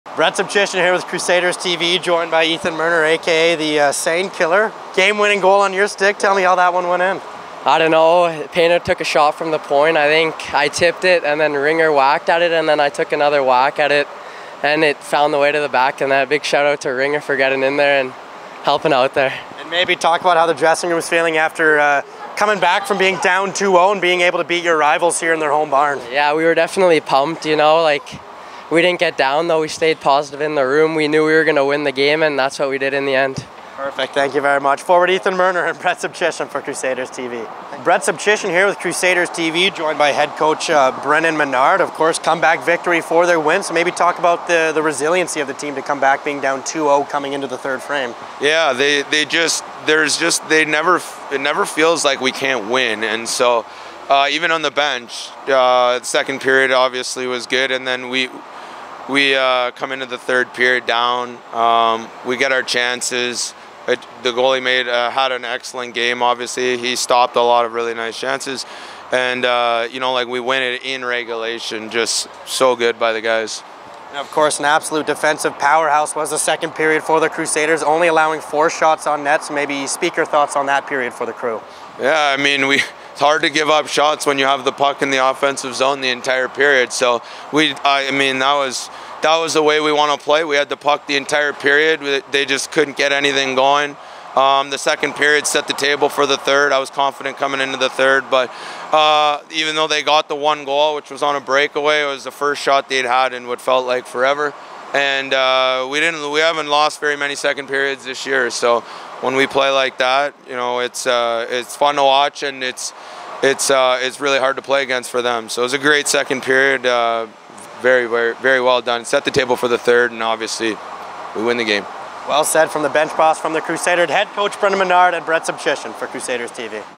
Postgame Interview - March 1st vs Spruce Grove
Brett Sobchyshyn speaks with forward Ethan Merner and head coach Brennan Menard following our 3-2 victory over the Spruce Grove Saints.